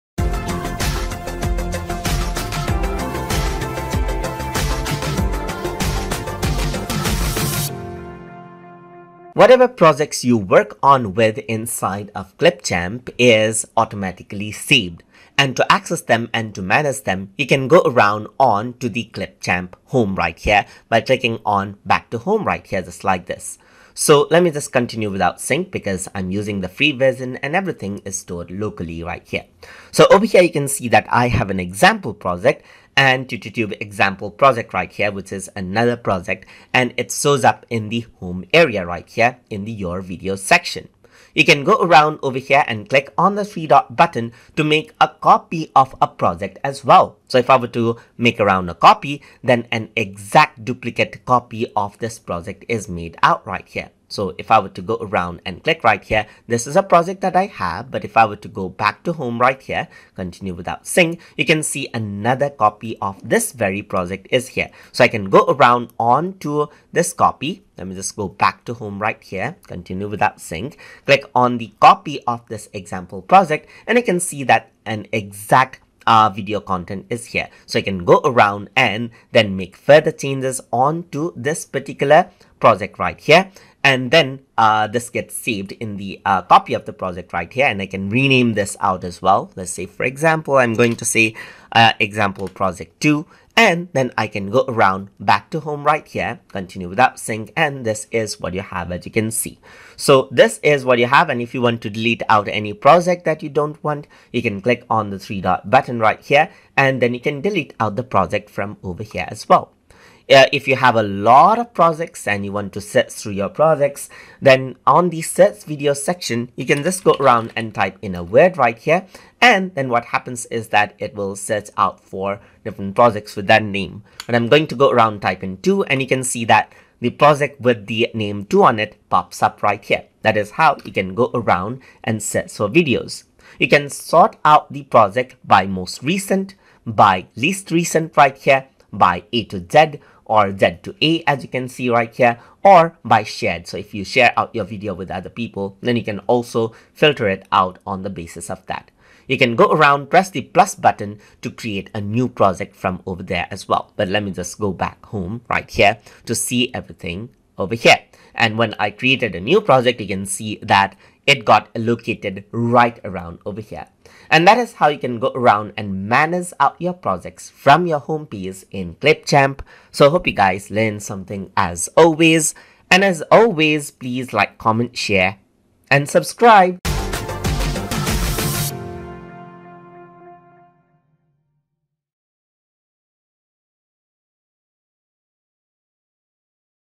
Clipchamp Tutorial - Lesson 10 - Managing Projects from Homepage
In this tutorial, we will be discussing about Managing Projects from Homepage in Clipchamp

Learn the basics of using Clipchamp including how to cut, edit, add effects and export videos.

This lesson has been made using Clipchamp version 2.5.15 but is applicable for newer and older versions of the application as well.